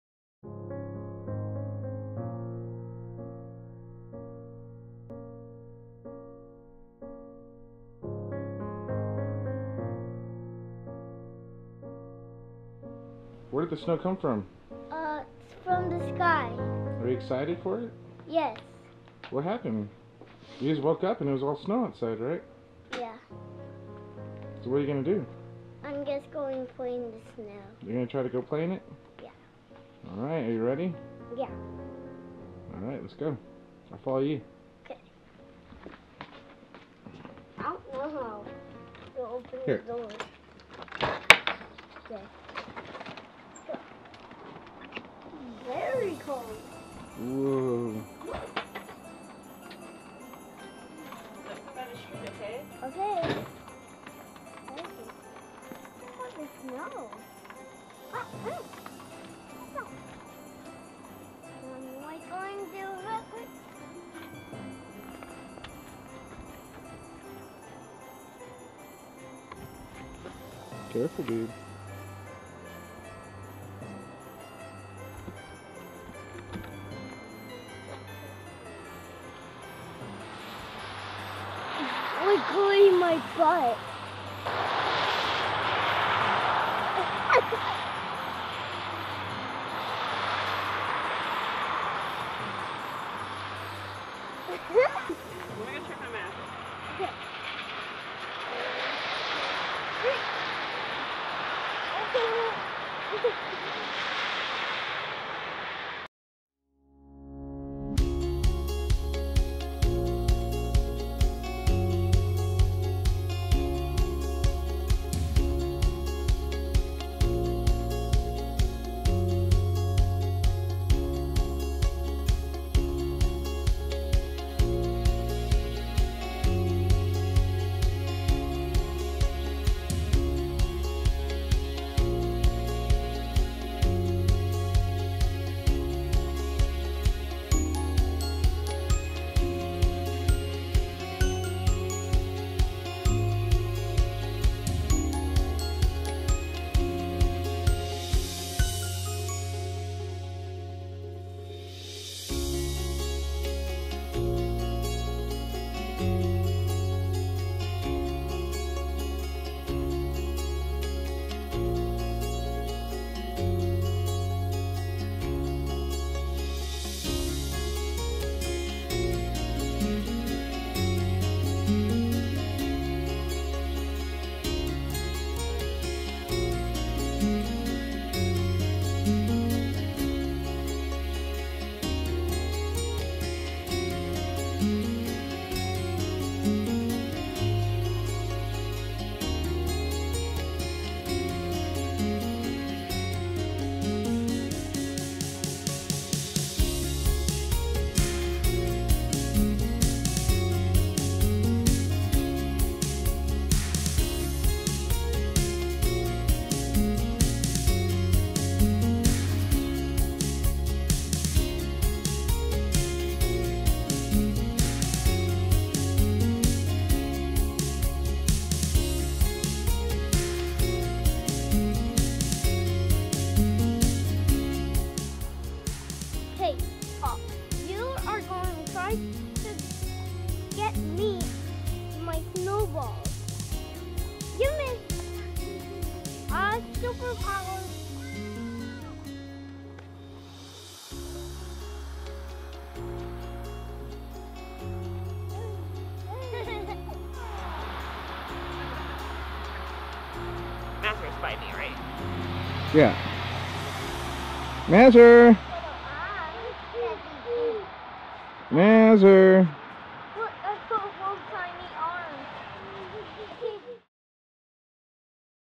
Snow Day in Corpus Christi Texas December 9th 2017
It snowed for the first time in 14yrs in Corpus Christi, the boys had a blast playing in the snow!